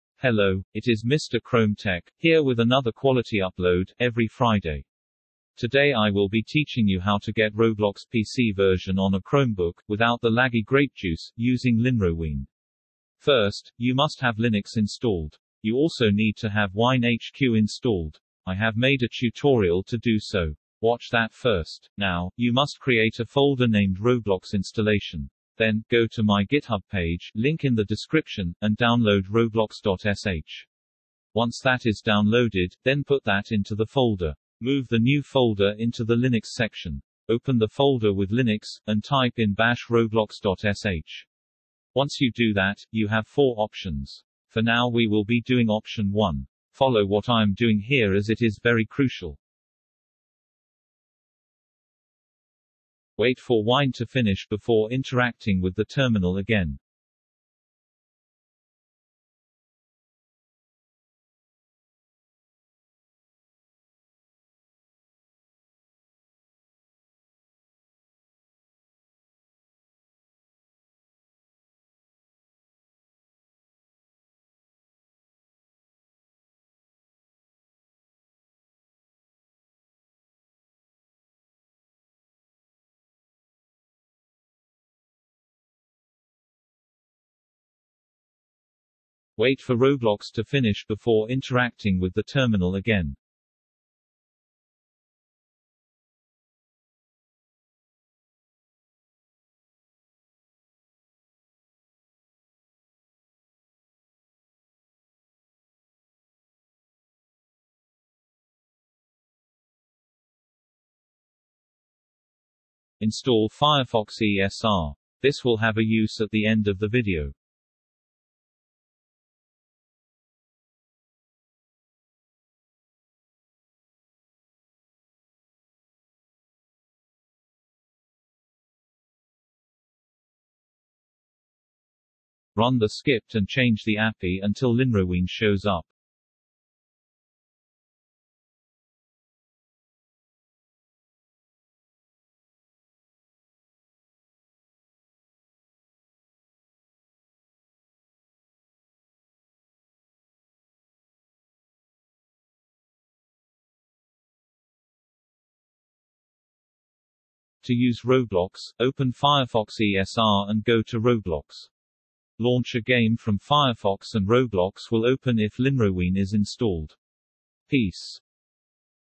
Install PC Roblox on Chromebook (No GrapeJuice 2022)
Today I will be teaching you how to install roblox pc version on a chromebook with LINROWINE! (Made by me)